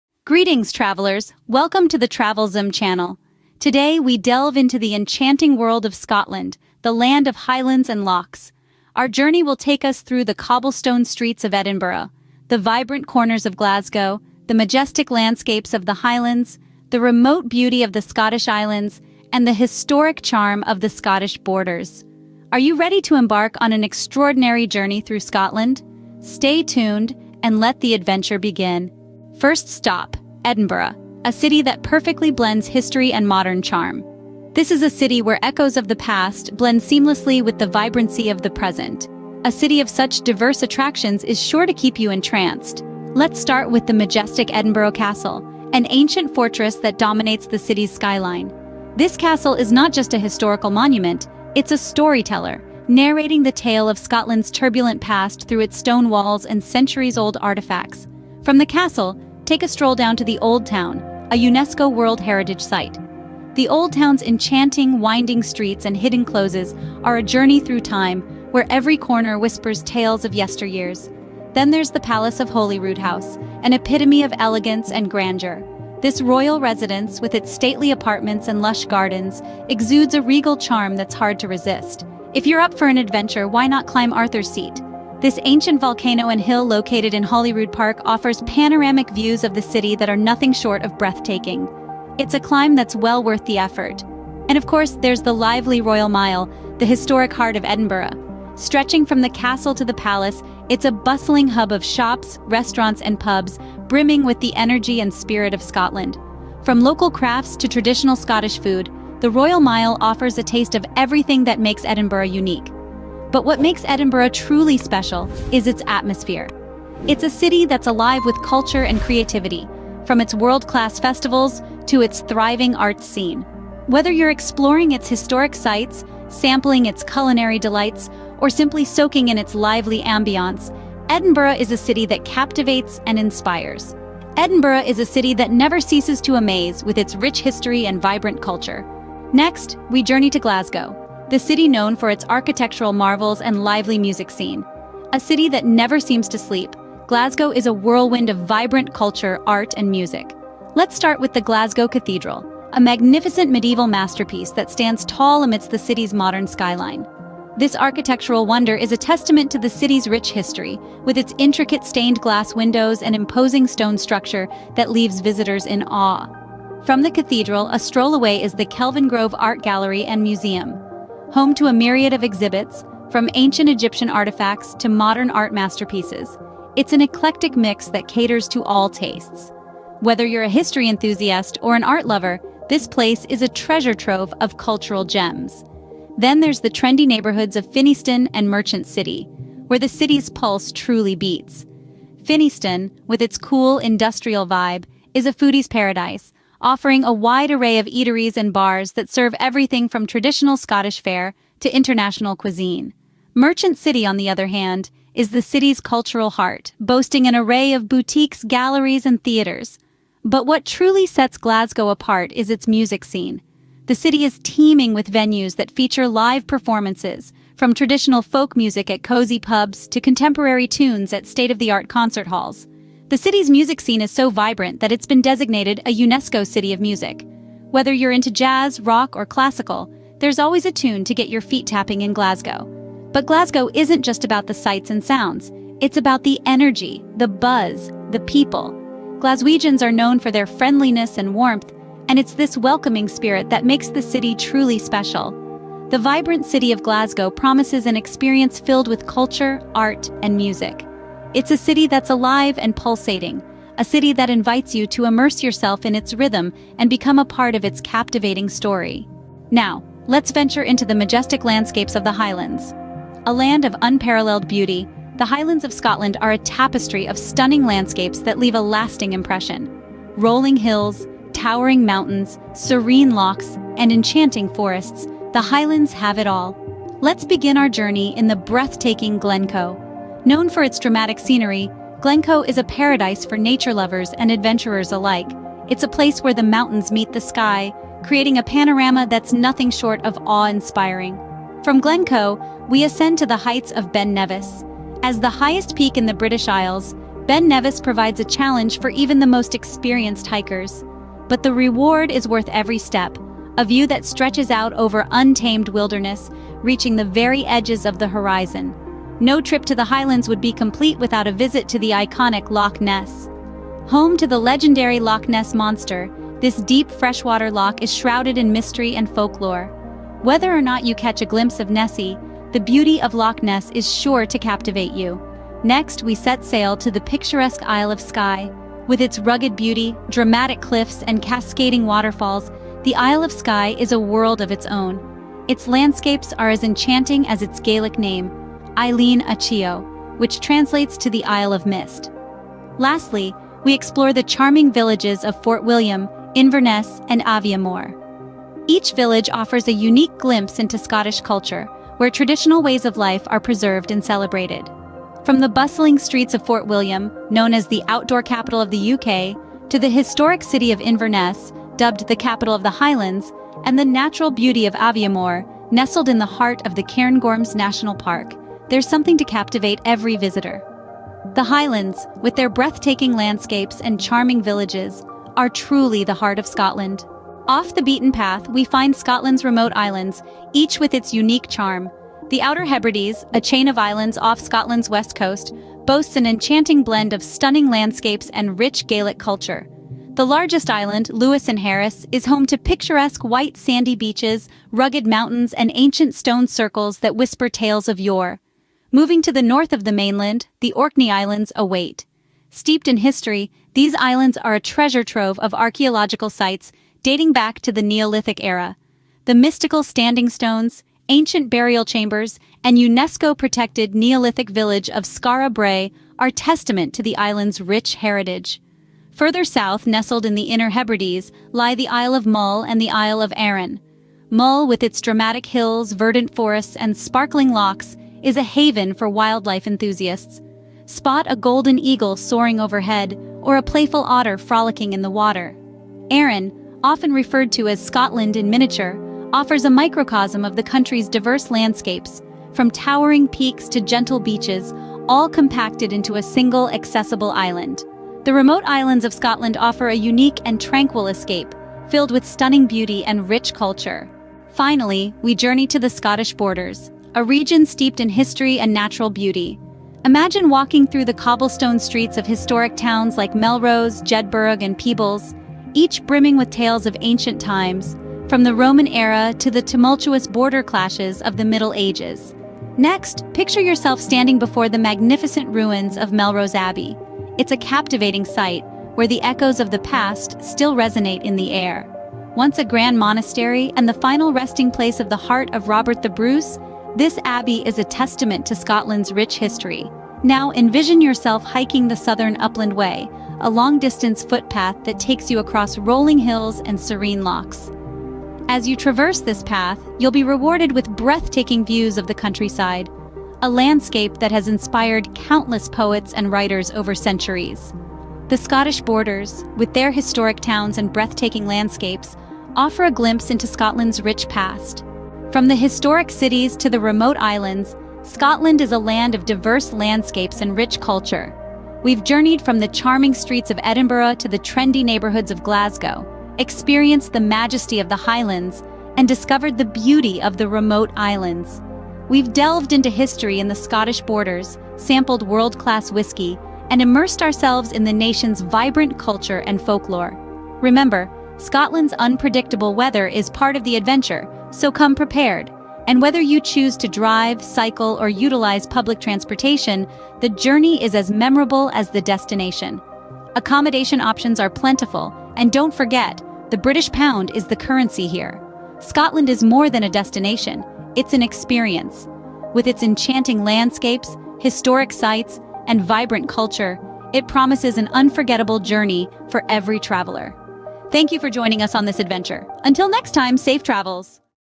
scotland ultimate travel guide 2024 - the incredible land of highlands and lochs
Explore the beauty and charm of Scotland in 2024 with our ultimate travel guide! From the historic cities of Edinburgh and Glasgow to the stunning Highlands and serene islands, this video covers it all. Get ready to embark on a journey filled with landscapes that will leave you speechless and experiences that will stay with you forever.

Join us as we delve into the rich history, vibrant culture, and breathtaking scenery that make Scotland a must-visit destination. Learn practical tips and insider information to make your trip smooth and unforgettable. Whether you're a history buff, outdoor enthusiast, or simply seeking new adventures, Scotland has something for everyone.

Don't miss out on this comprehensive guide that will help you plan the perfect Scottish getaway in 2024.

00:00:00 Welcome to the Land of Highlands and Lochs
00:00:29 The Charm of Edinburgh
00:02:19 The Vibrant Glasgow
00:04:19 The Majestic Highlands
00:06:17 The Remote Islands
00:07:34 The Historic Scottish Borders
00:08:35 Wrapping up the Scottish Adventure